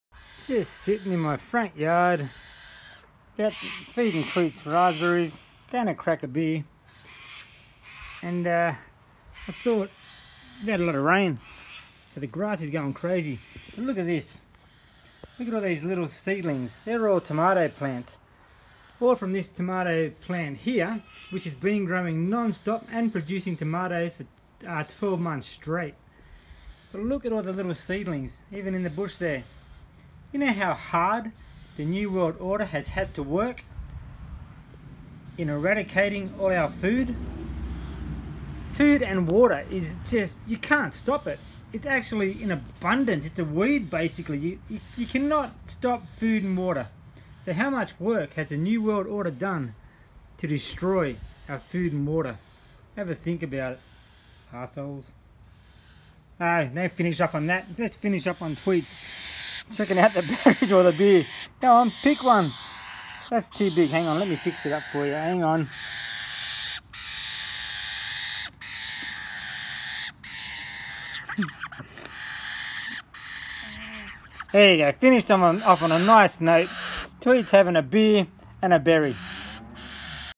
Beers, Berries & Tweets. And how earth provides a Bundle of food to eat! So where is it?
This was at 6.30pm yesterday.

Tweets came home this morning with a potentially broken wing and can’t fly right now, so I’ve got her in the garage safe and still happy.

She’s still eating and in good spirits, there no damage to her so I doubt she was attacked..

It’s funny how few people know that over 80% of their “food” is genetically modified!

It’s not the food “God” gave us.

When you see first hand how many plants grow from seeds falling on the ground each year, and just grow without any human intervention, it’s impossible for humanity to run out of organic food... so why have we???